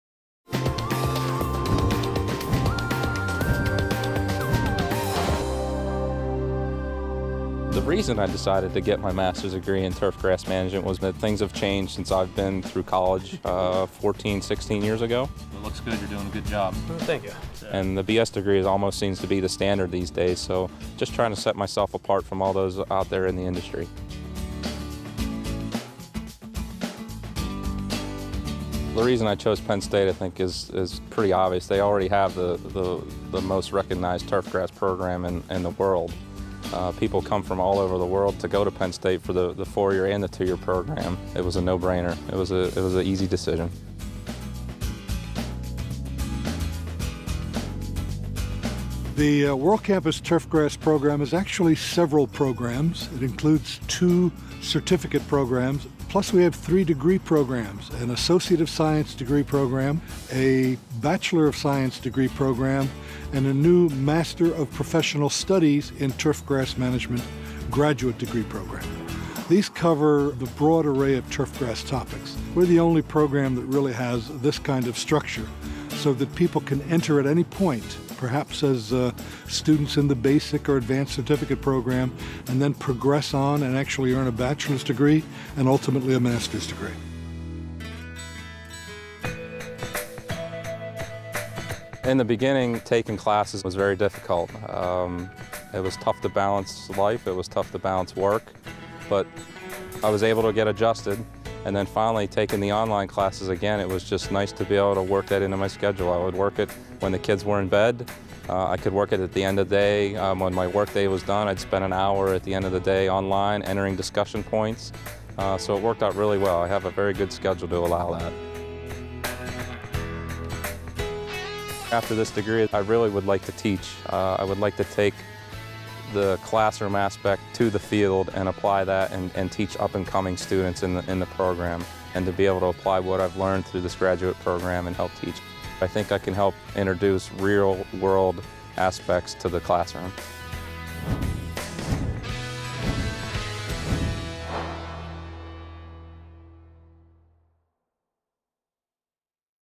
Penn State Turfgrass Degree Programs Online
You can earn your turfgrass degree online through Penn State World Campus.

Look inside our turfgrass science and turfgrass management programs through the eyes of current students. See what other students are saying about their experiences in the program and why they chose Penn State.

Earn your Penn State degree, anywhere, anytime, online!